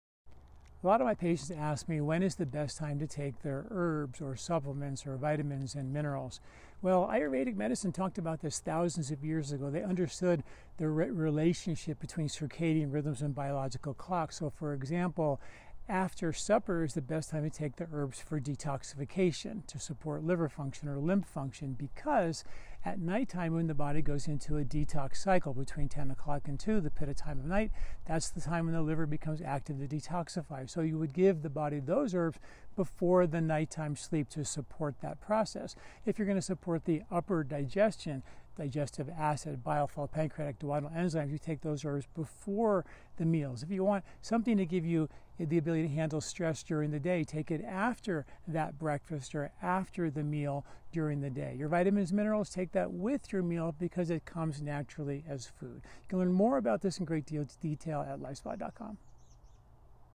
Best Time to Take Herbs, Supplements & Vitamins!
When do you take your medicine? 💊 Most of us just follow the label, but what if the time of day matters more than the dose?

The science of Circadian Medicine—validated by a Nobel Prize—confirms what Ayurveda has known for thousands of years: Your body's internal clock dictates when drugs and herbs are most effective and least toxic. ⏰

Stop ignoring your inner clock! Learn the best time to take your medicine (and your herbs) using Circadian Wisdom. 🔗